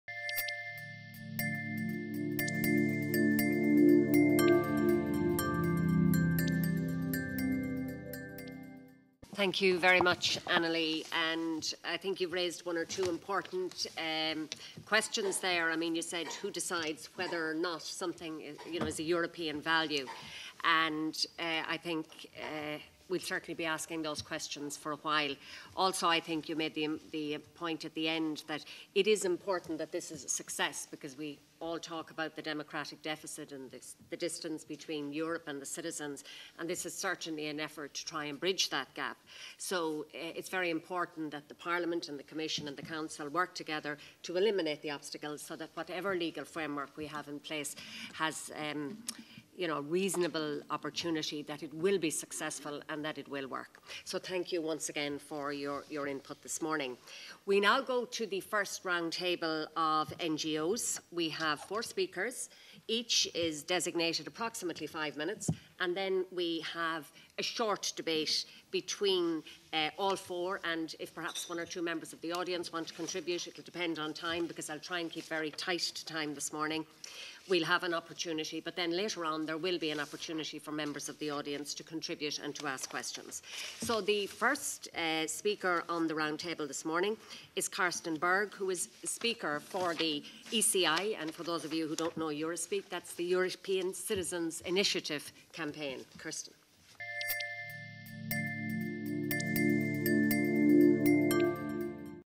[SPEV] Marian Harkin - European Citizens' Initiative
Marian Harkin MEP (ALDE ADLE Member) - Intervention at the Seminar: European Citizens' Initiative - Questions of Implementation

[SPEV] [MEP Speech in Event] [Video Statement] [Video] [Seminar] [Language EN]

100622-ALDEADLE-MEP-SPEECH-IN-EVENT-Marian_Harkin-Seminar-on-European_Citizens_Initiative